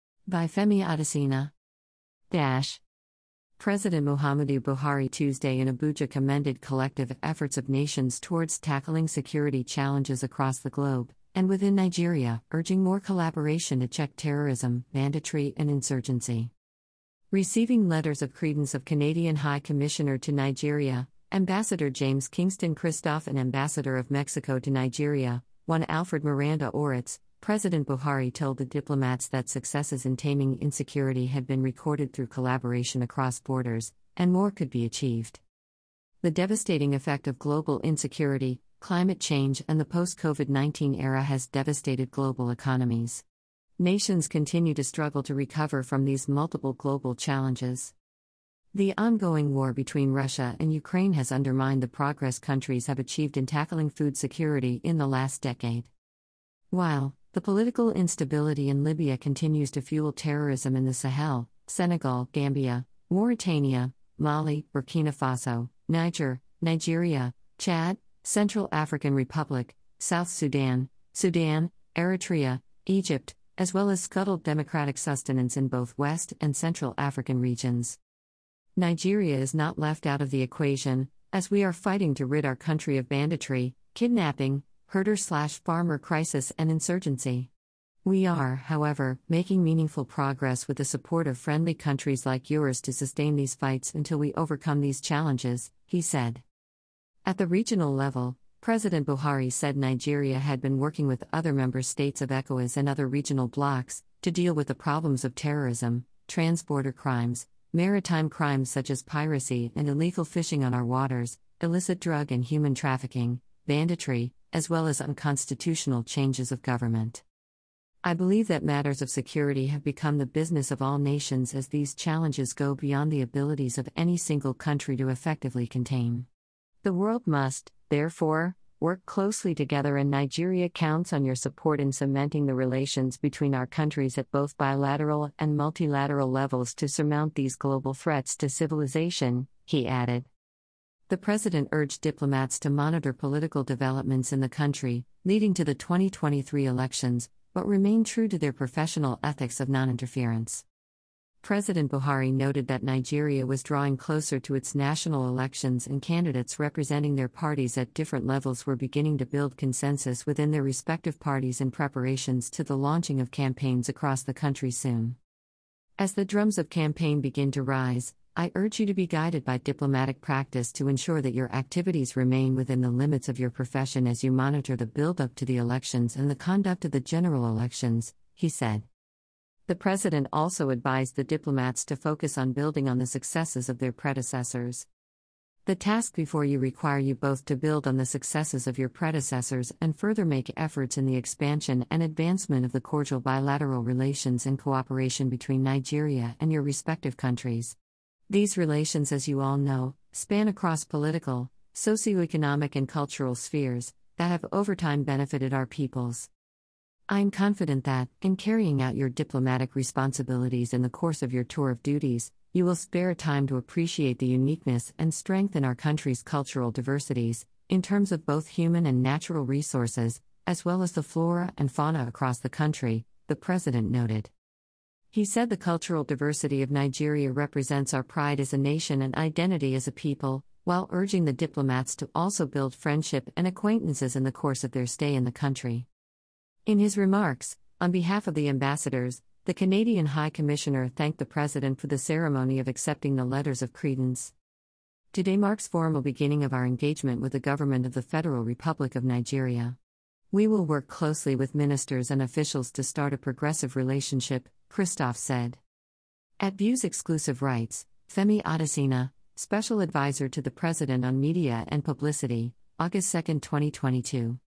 Political Instability In Libya Fuels Terrorism In Nigeria - Buhari ~ OsazuwaAkonedo
Political Instability In Libya Fuels Terrorism In Nigeria - Buhari ~ OsazuwaAkonedo Muhammadu #Abuja #Adesina #African #Buhari #Commissioner #Femi OsazuwaAkonedo By Femi Adesina